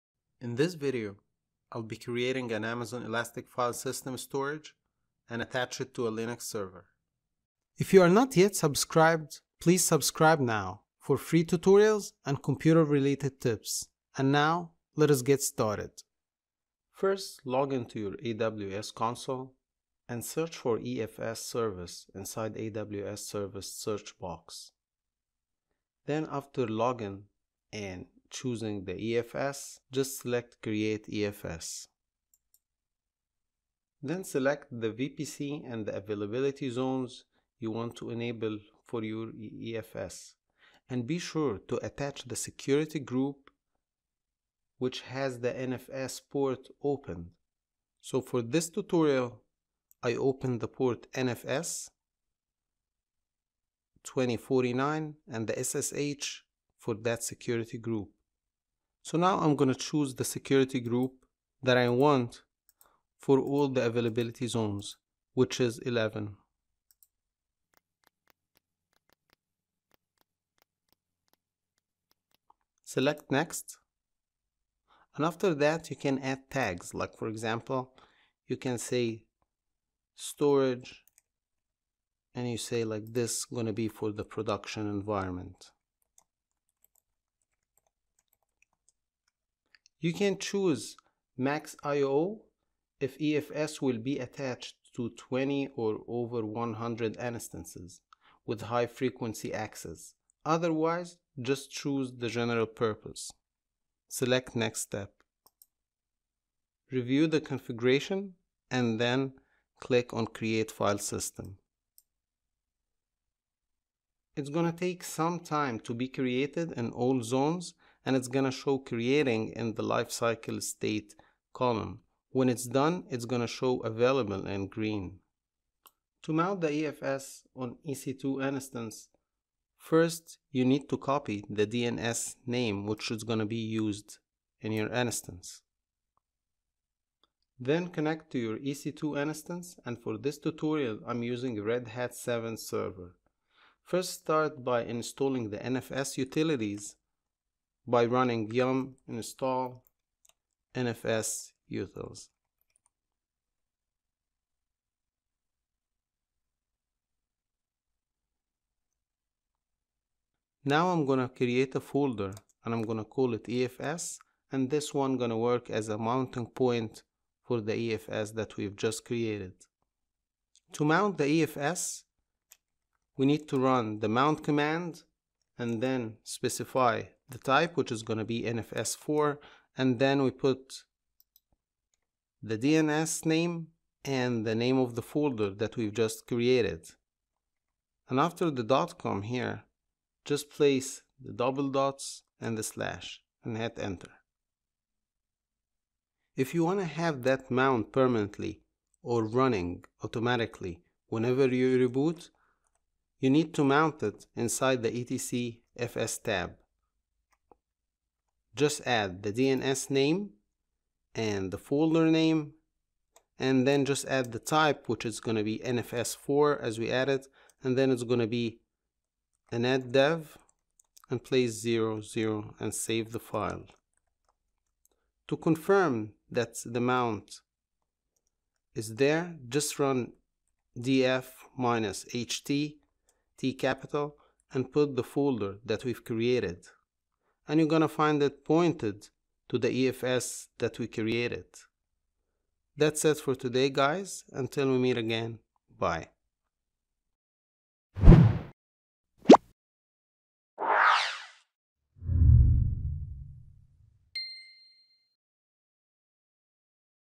AWS EFS Tutorial: Create EFS and Mount it on Linux
AWS Elastic File System Tutorial
In this tutorial i will be creating an AWS EFS and mount the efs on Linux.

What is EFS?

Amazon Elastic File System or AWS EFS provides a very simple out of the box scalable file storage, the size of efs is increasing and decreasing automatically based on the need and your usage, you can mount that storage type on multiple of EC2 servers which is not the case with other storage.

Create EFS And Mount It

Create EFS

Login to your console
Search for EFS service inside AWS services search box
Select 'create EFS'
Select VPC and the availability zones you want to enable for your EFS 'Be sure to attache a security group which has the NFS port opened'.
Add tags if needed.
Choose MAX I/O if EFS will be attached to 20-100+ instances with high frequency access otherwise choose general purpose.
Review configuration and select 'Create File System'.
It takes sometimes to be created in all zones, once it's done it will show green 'Available' text under 'Life cycle state' column.
Copy the DNS name which will be used for mounting purpose.
Mount EFS on EC2 instance

Connect to your EC2 instance "For this tutorial i am using a RedHAT 7 box"
Install nfs utilities
yum -y install nfs-utils
Create a folder to use it as an EFS mounting point.
mkdir /var/www/mymount
Run below mount command by replacing both the folder name and the DNS name with the ones you have.
Confirm the mount

df -hT /home/ec2-user/myMount

Above command should give an output shows that folder is pointed and mounted to the EFS using nfs4.

To mount the EFS permenantly or have it mounted when system is rebooted you need to add the mount to you /etc/fstab

vi /etc/fstab
fs-xxxxxxxxxx.efs.us-east-1.amazonaws.com:/ /home/ec2-user/myMount nfs4 _netdev 0 0